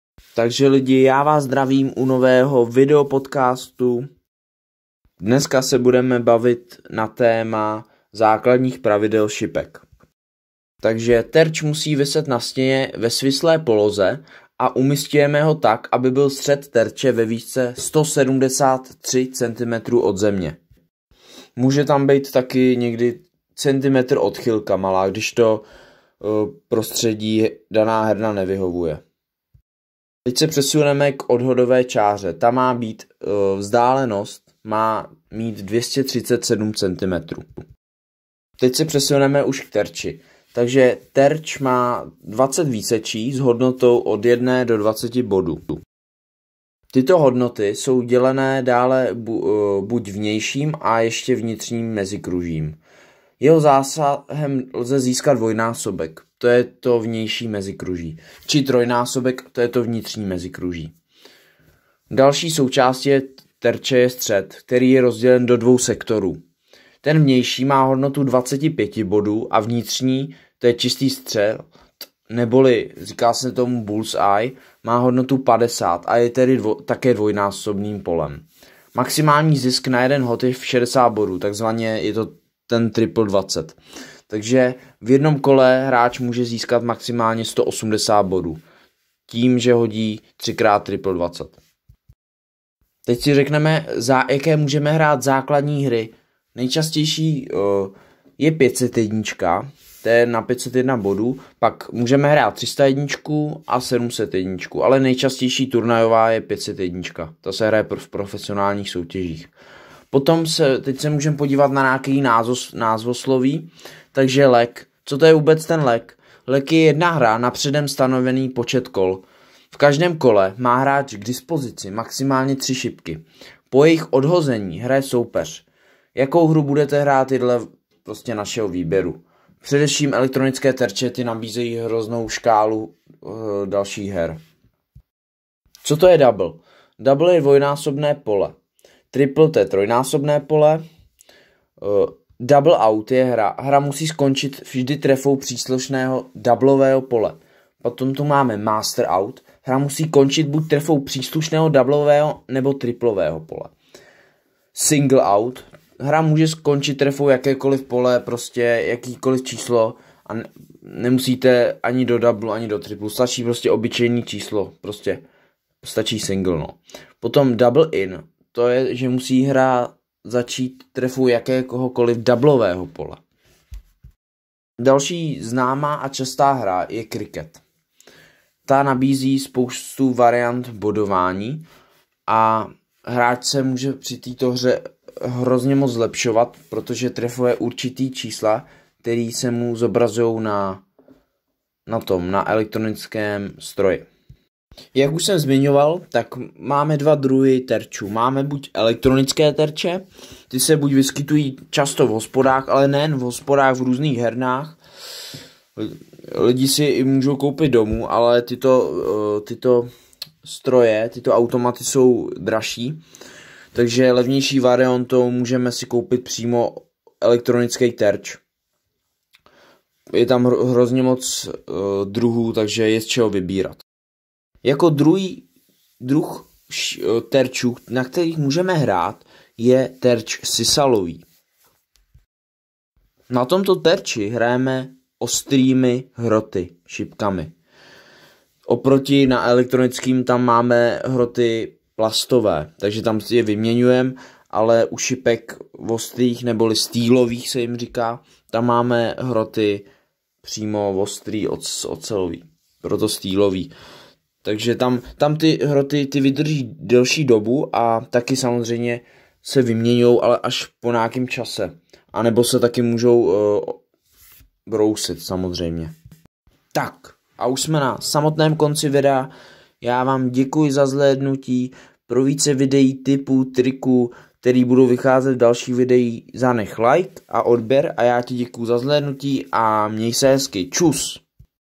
Základní pravidla šipek - podcast
Dnes se podíváme na základní pravidla šipek!